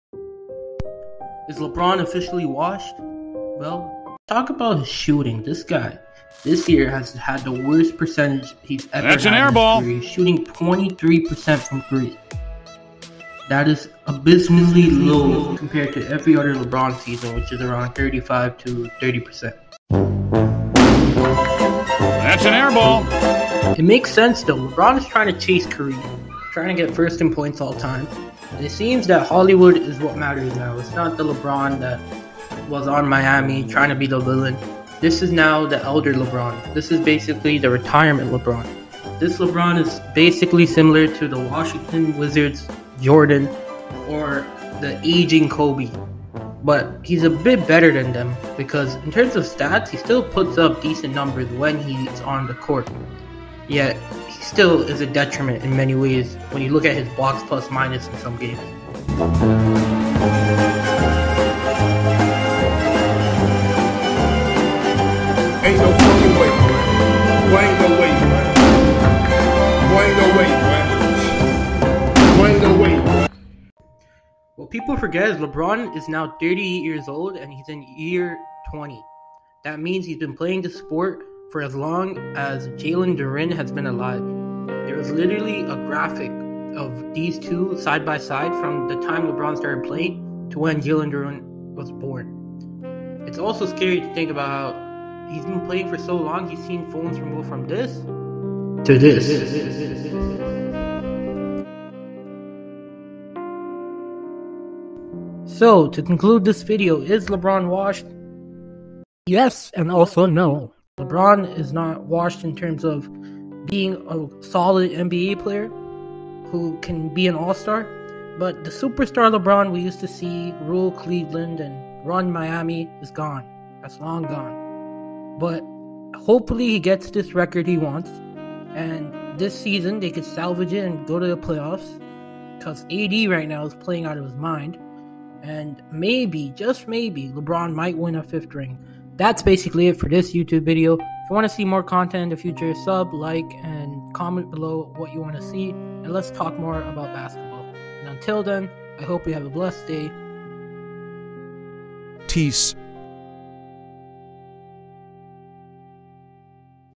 Is Lebron Officially Washed? (This Is Not Looking Good...)
This video is just me talking about this being LeBron's worst NBA season up to date, and how he may be finally showing signs of aging. However it is to be expected with his age as he is in year 20.
Thanks!